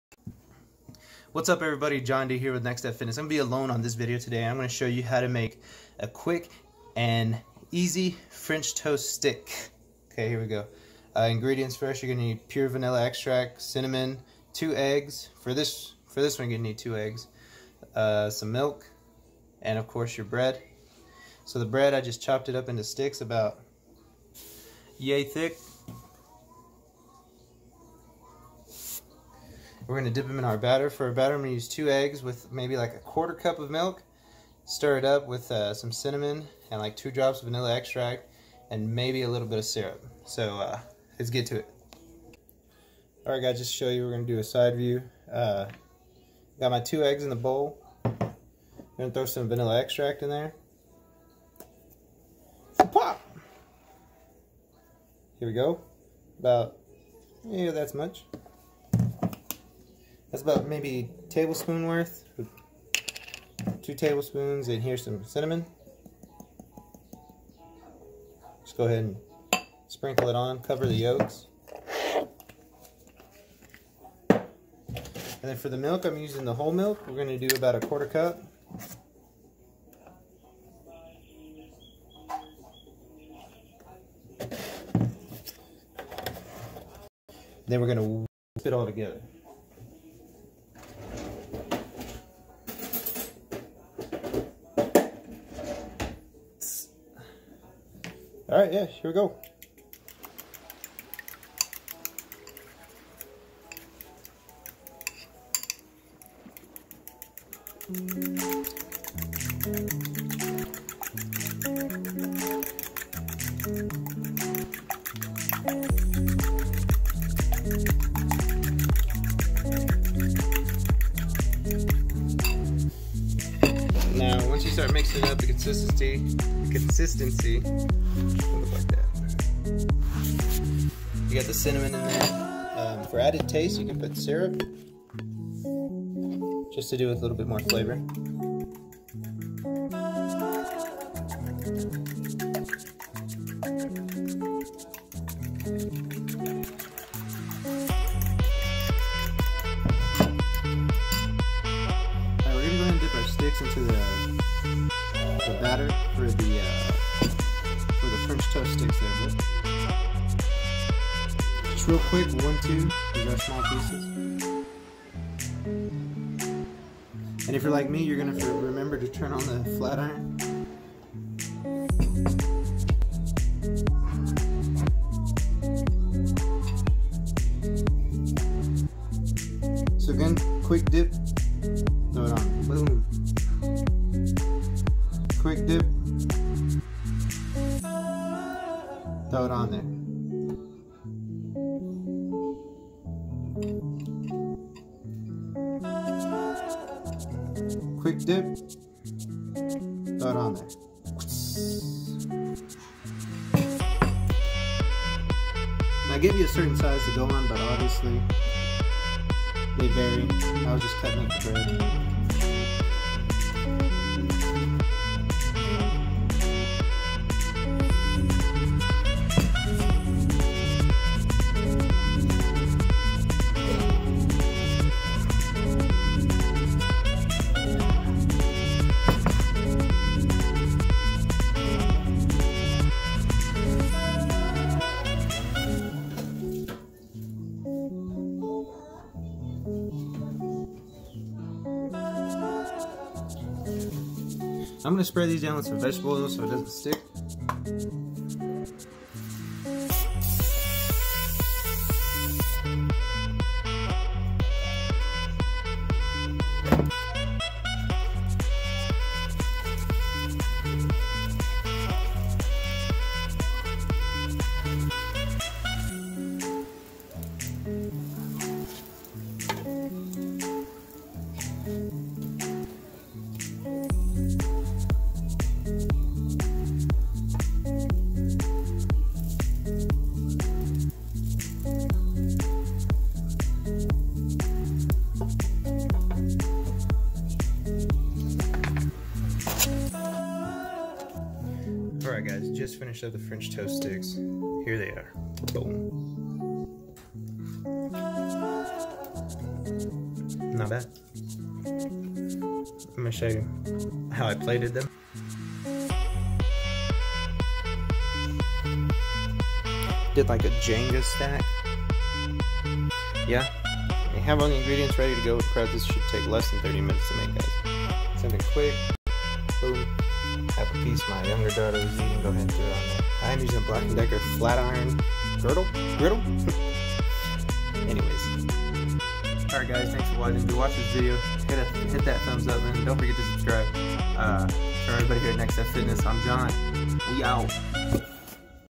How To: Fastest/easiest French Toast sticks!
We made french toast sticks. Because of america needs that right now with the holidays going on and Christmas and the relelection.